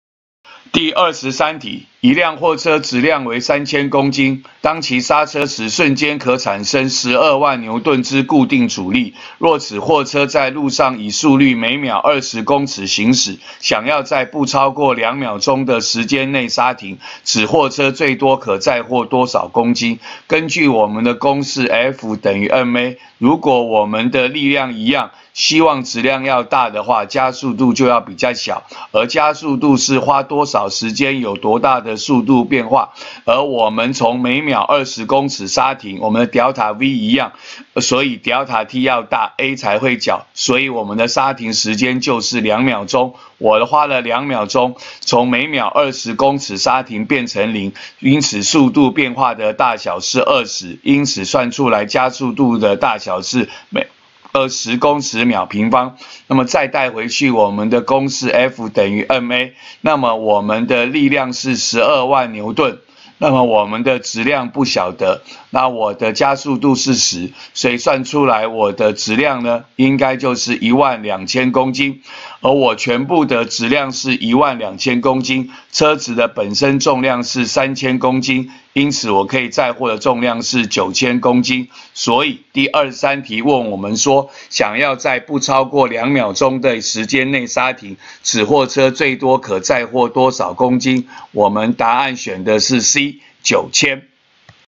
106學年上9年級理化二段第23題
給貨車質量、速率，及剎車力量要在一定時間內剎停，問最多可載貨多少公斤。